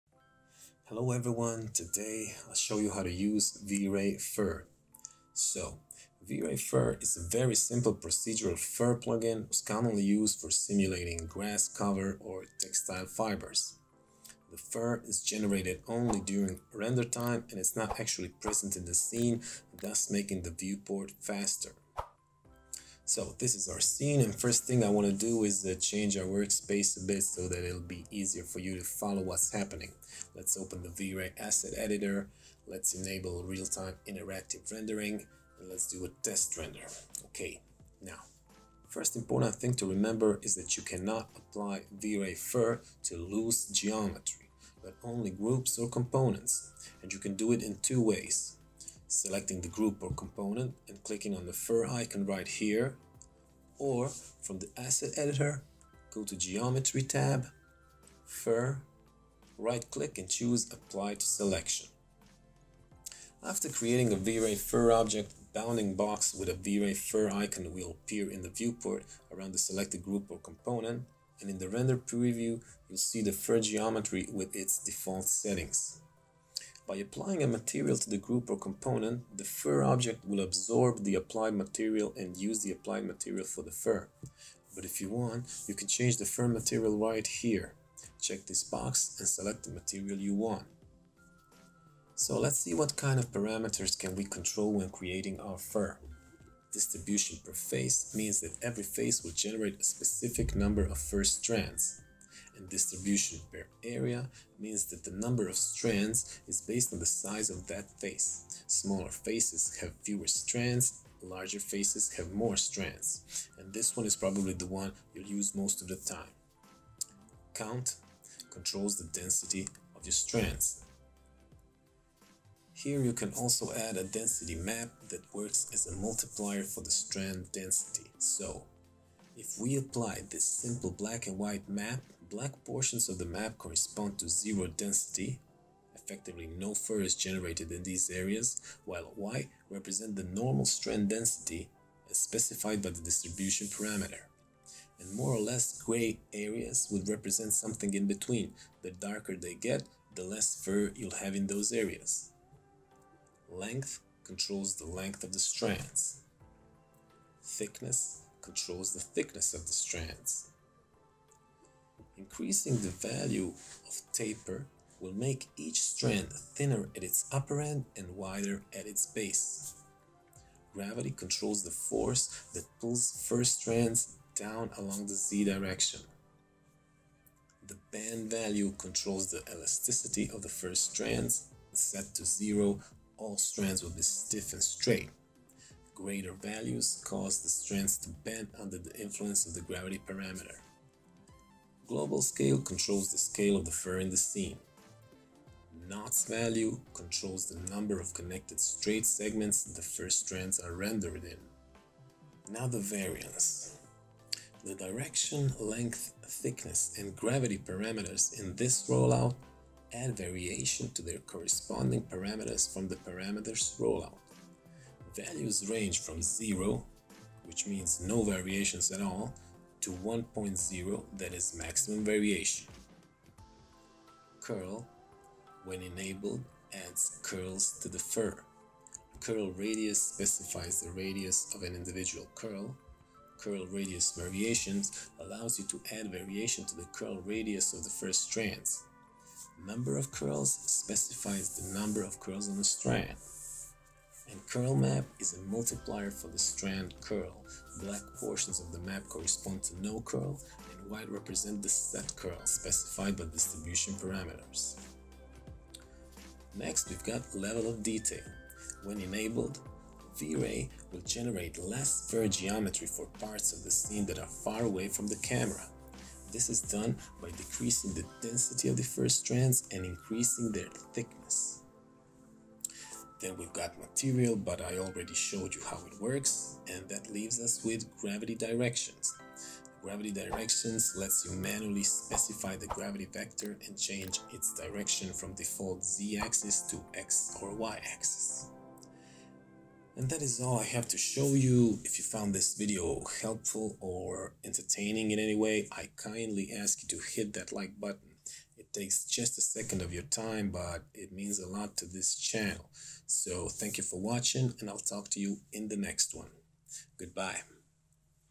VRay Fur tutorial - Create grass in a matter of seconds
V-Ray Fur is a very simple procedural fur plugin most commonly used for simulating grass cover or textile fibers.
 The fur is generated only during render time and is not actually present in the scene, thus making the viewport faster.
After creating a V-Ray Fur object, a bounding box with a V-Ray Fur icon will appear in the viewport around the selected group or component. And in the render preview you’ll see the fur geometry with its default settings
By applying a material to the group or component, the fur object will absorb the applied material and use that applied material for the Fur.